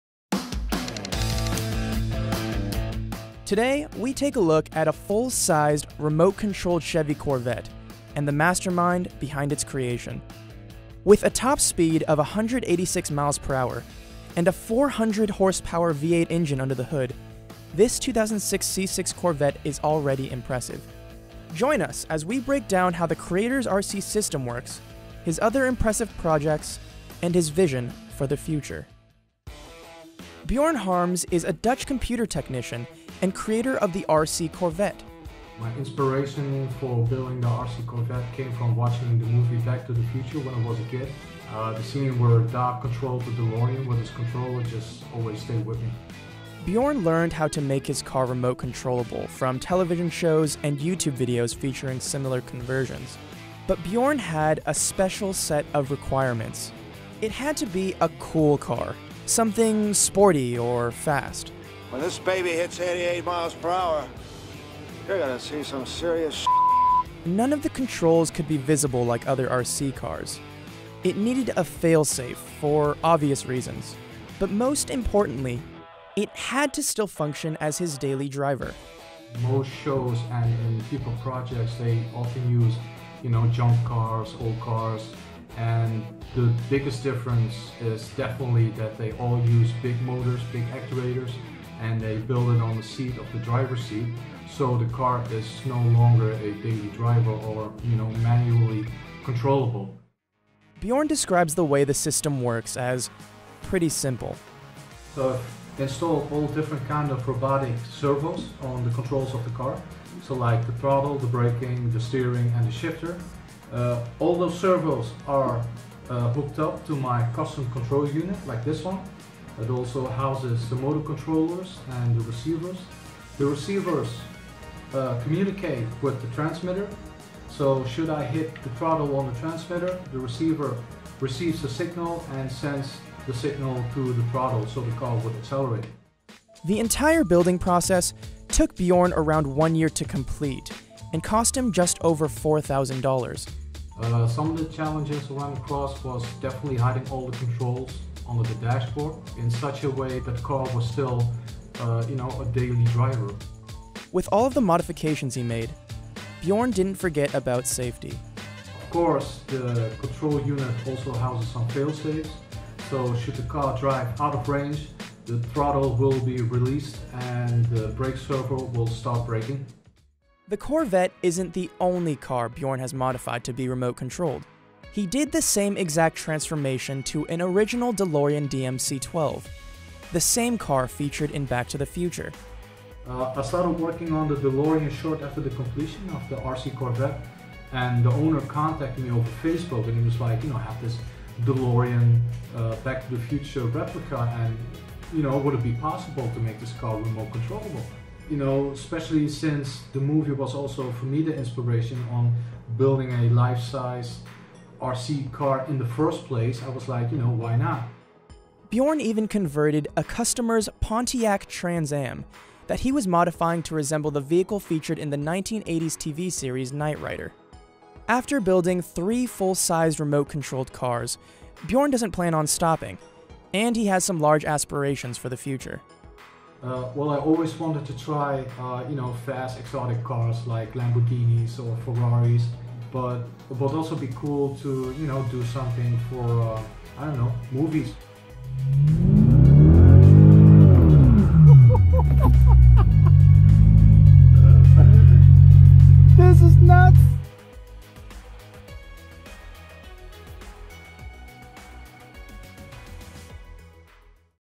Life Size Remote Controlled Corvette
With some inspiration from Doc Brown, Dutch tech whiz Bjorn Harms turned his sixth-generation 'Vette into a fully remote-controllable sports car.

Tech Insider tells you all you need to know about technology: technology car, super car, gadgets, how-to's, gaming, science, digital culture, and more.